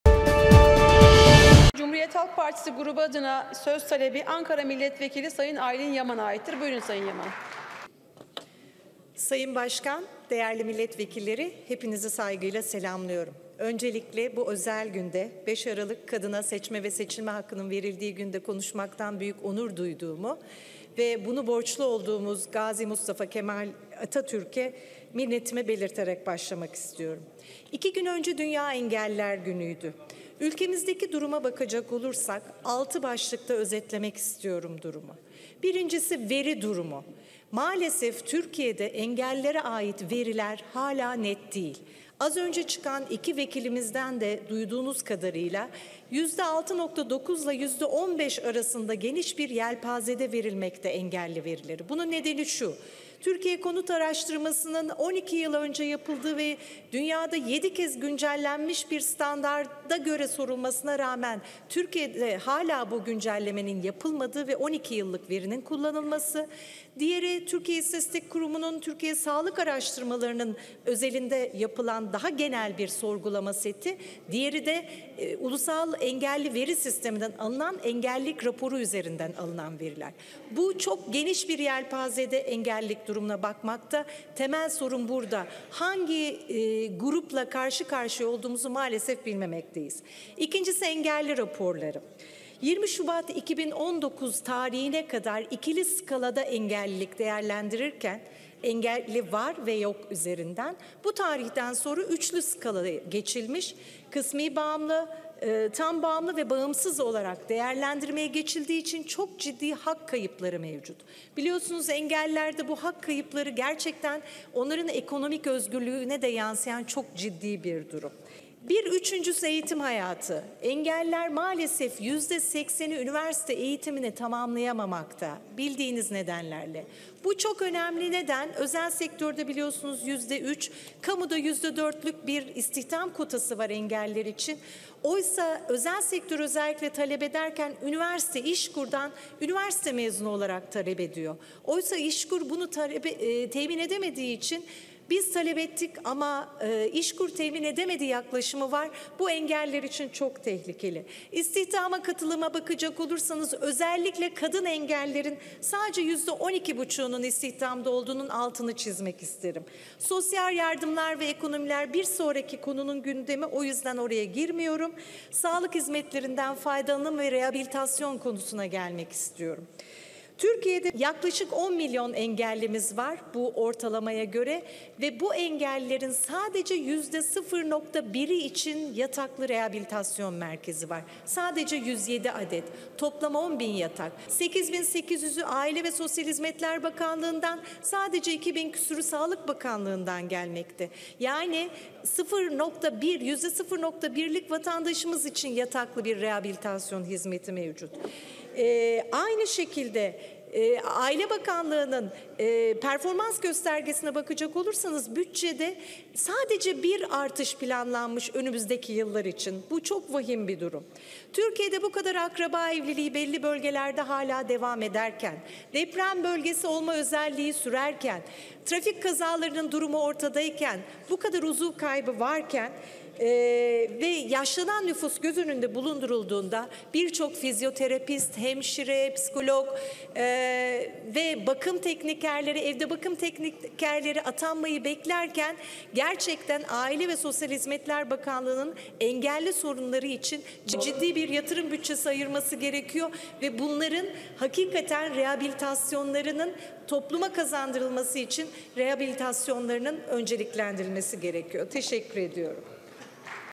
Aylin Yaman Engelli sorunları için gerçekten ciddi bir yatırım bütçesi ayırması gerekiyo
3 Aralık Dünya Engelliler Günü kapsamında, TBMM Genel Kurulu'nda "Engellilerin sorunları ve çözüm yollarının araştırılması" grup önerisi üzerine konuşma yapan Sağlık, Aile, Çalışma ve Sosyal İşler Komisyonu üyesi ve Cumhuriyet Halk Partisi Ankara Milletvekili Dr. Aylin Yaman; “Aile ve Sosyal Hizmetler Bakanlığının engelli sorunları için gerçekten ciddi bir yatırım bütçesi ayırması gerekiyor. Engellilerin rehabilitasyonlarının önceliklendirilmesi gerekiyor” değerlendirmesinde bulundu.
Genel Kurul konuşmasına, ülkemizdeki engellilerin durumuna dair genel bir değerlendirme ile başlayan CHP’li Yaman, “Engellilerin durumunu altı başlıkta, istatistiki veri tabanı, engellilik raporu, eğitim hayatı, istihdama katılım, sosyal yardımlar ve ekonomik durum ve son olarak engellilerin sağlık hizmetleri ve rehabilitasyon süreçlerinden faydalanımı şeklinde özetlemek mümkün olur. Birinci konu, veri durumu. Maalesef, Türkiye'de engellilere ait veriler hâlâ net değil. Bugün için yüzde 6,9 ile yüzde 15 arasında geniş bir yelpazede verilmekte engelli verileri. Bunun nedeni; on iki yıl önce DSÖ’nün soru seti kullanılarak yapılan Türkiye Nüfus ve Konut Araştırması'ndan elde edilen verilerin kullanılması ve dünyada yedi kez güncellenmesine rağmen hala o verilerin gündemde olmasıdır. Diğeri ise, Türkiye İstatistik Kurumunun Türkiye sağlık araştırmaları özelinde yapılan daha genel bir sorgulama setidir. Bir diğeri ise Ulusal Engelli Veri Sistemi'nden alınan engellilik raporu üzerinden alınan verilerdir. Bu veri sistemleri çok geniş bir yelpazede engellilik durumuna bakmakta, temel sorun da buradan kaynaklanmaktadır. Ne büyüklükte bir grupla karşı karşıya olduğumuzu maalesef bilmemekteyiz” dedi.
İkinci bir başlık olarak engelli raporlarına değinen Dr. Aylin Yaman, “20 Şubat 2019 tarihine kadar engellilik "engeli var" ve "engeli yok" üzerinden ikili skalada değerlendirilirken bu tarihten sonra üçlü skalaya geçilmiş "kısmi bağımlı" "tam bağımlı" ve "bağımsız" olarak değerlendirilmeye başlanmıştır. Bu süreç engelliler için çok ciddi hak kayıplarına neden olmuştur. Bilindiği üzere, engellilerde bu hak kayıpları onların ekonomik özgürlüğüne de yansıyan gerçekten çok ciddi bir durumdur” dedi.